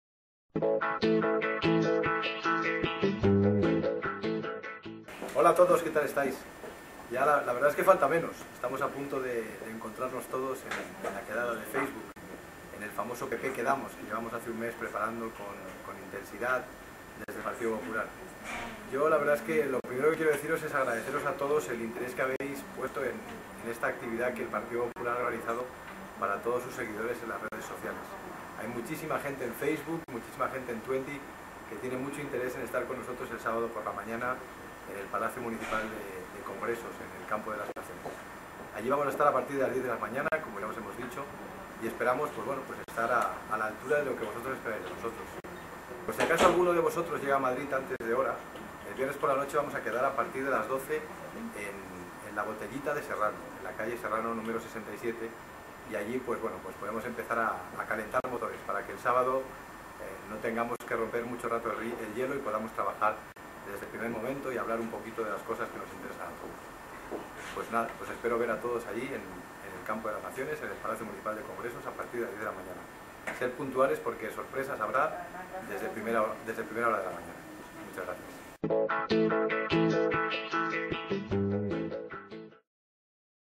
Queremos: José Luis Ayllón
José Luis Ayllón habla sobre QUEREMOS, I Foro Abierto de militantes del Partido Popular